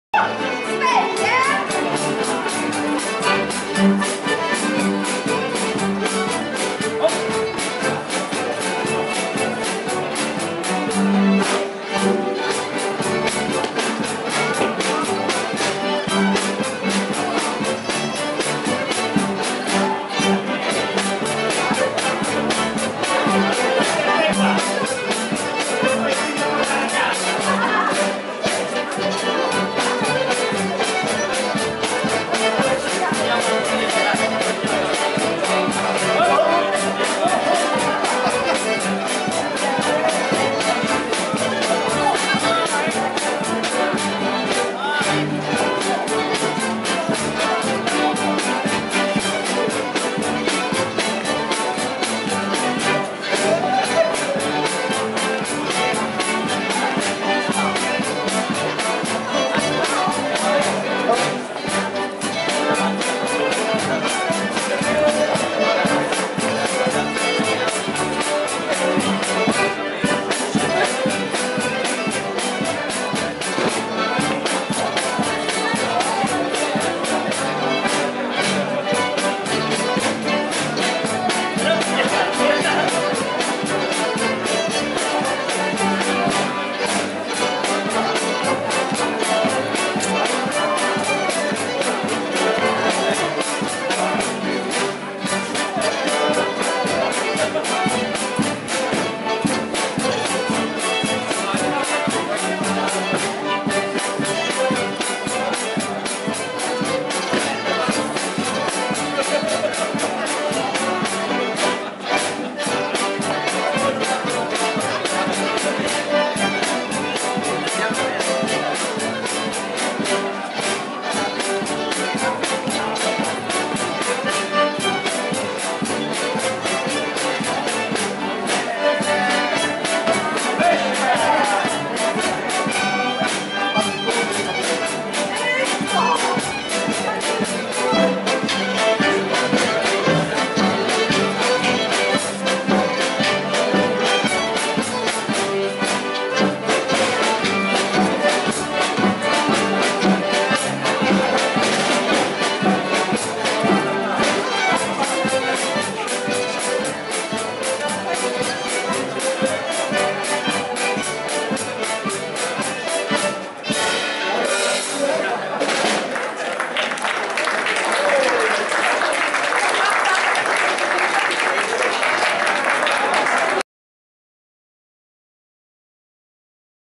Contradanza, antico ballo montanaro, Monghidoro 2014.
Contradanza, ballo antico montanaro eseguito dai Suonatori della Valle del Savena e ballato dai ballerini Balli Montanari, alla Tartufesta a Monghidoro (Appennino Bolognese Italy) 1° novembre 2014. Riprese di Gilberto Tedeschi.